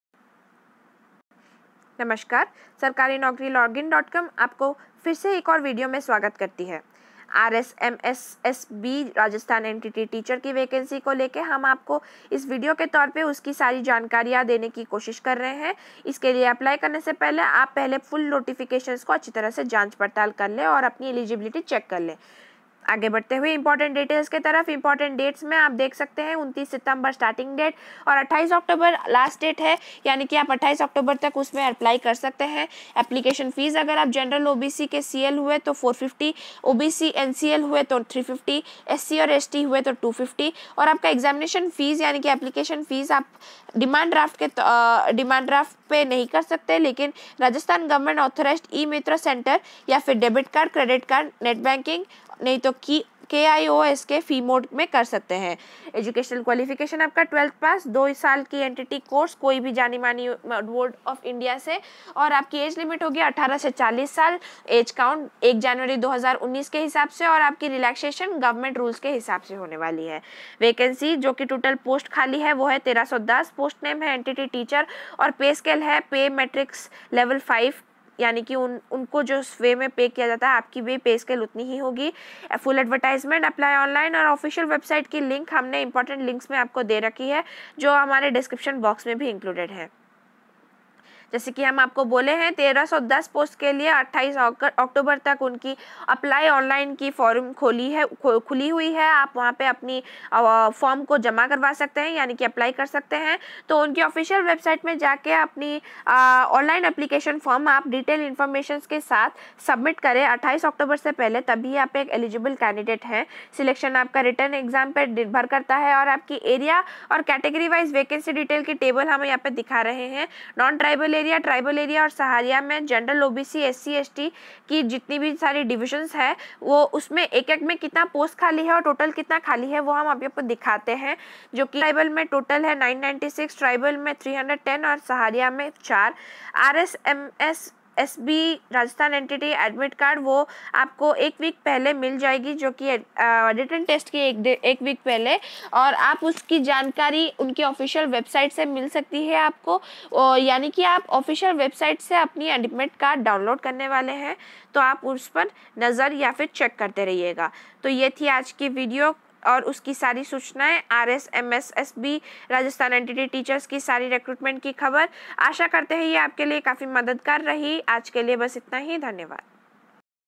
RSMSSB Rajasthan NTT Teacher Vacancy 2018
RSMSSB Rajasthan NTT Teacher Vacancy 2018 – Online application forms are invited by Rajasthan Subordinate and Ministerial Service Selection Board for the recruitment of NTT Teacher vacancy in Rajasthan. Interested candidates read full notification for eligibility before apply online for RSMSSB Rajasthan NTT Teacher Vacancy 2018.
RSMSSB Recruitment 2018
Rajasthan Rajasthan NTT Teacher Vacancy 2018

Important Details
Important Dates Application Fee
 Start Date: 29 Sep 2018
 Last Date: 28 Oct 2018
 For more details check the notifications.  For Gen/OBC CL: Rs. 450/-
 For OBC NCL: Rs. 350/-
 For SC / ST / Rs. 250/-
 Pay the exam fee through cash at Rajasthan
Govt. Authorized E MitraCenter or Debit Card,
Credit Card, Net Banking KIOSK Fee mode.
 For more details check the notifications.
Education Qualification Age Limit
 12th Pass
 2 Years NTT Course
 From recognized board of India
 For more details check the notifications.  Minimum Age: 18 years
 Age Count On: 01 Jan 2019
 Age relaxation as per Govt rules.
Vacancy Details
 Total Post : 1310
 Post name : NTT Teacher
 Pay Scale : pay Matrix Level 5
Important Links
Full Advertisement Cilck Here

Rajasthan Subordinate and Ministerial Service Selection Board are inviting online applications for recruitment of 1310 post of  NTT Teacher in Rajasthan. Interested and eligible candidate can apply online for Rajasthan NTT Teacher vacancy from 29 Sep 2018 to 28 Oct 2018 on the official website of RSMSSB.
How to Apply for RSMSSB Rajasthan NTT Teacher Vacancy 2018:-
1. All the eligible candidates apply online only.
2. No other mode of submission of application would be accepted.
3. Read full notification for more detailed information.
4. Apply online from 29 Sep 2018 to 28 Oct 2018 on the official website of RSMSSB.
Selection Process of RSMSSB Rajasthan NTT Teacher Vacancy 2018:-
The selection Process of RSMSSB Rajasthan NTT Teacher vacancy is purly based on Written Exam.
 Written Exam
Area & Category Wise Vacancy Detail of RSMSSB Rajasthan NTT Teacher Vacancy 2018:-
Area GEN OBC SC ST Total
Non Tribal Area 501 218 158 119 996
Tribal Area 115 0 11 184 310
Saharia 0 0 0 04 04
RSMSSB Rajasthan NTT Teacher Admit Card 2018:-
All candidate who applied for Rajasthan NTT Teacher can download their Admit card from the official website of RSMSSB from a week before Written Exam.